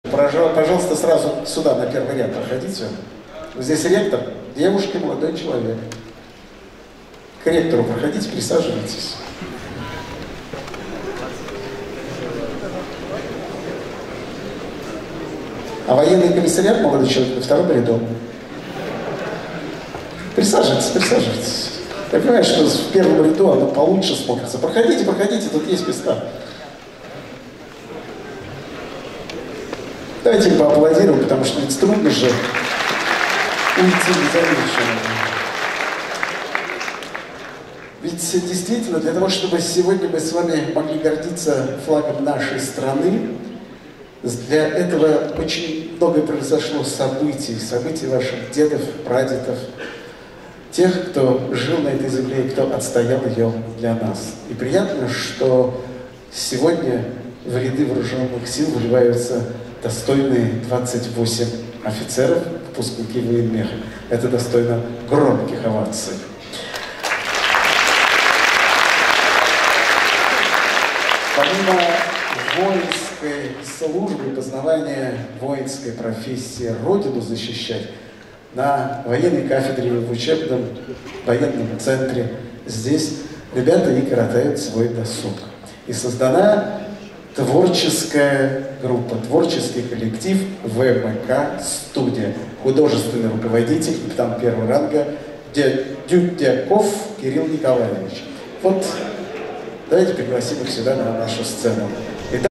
5 марта в актовом зале БГТУ «ВОЕНМЕХ» им. Д.Ф. Устинова состоялось торжественное мероприятие, посвященное выпуску офицеров, закончивших обучение в Учебном военном центре. Это

уже 5-й выпуск молодых специалистов УВЦ. Поздравить и поддержать выпускников пришли студенты, профессорско-преподавательский состав, родители и гости университета.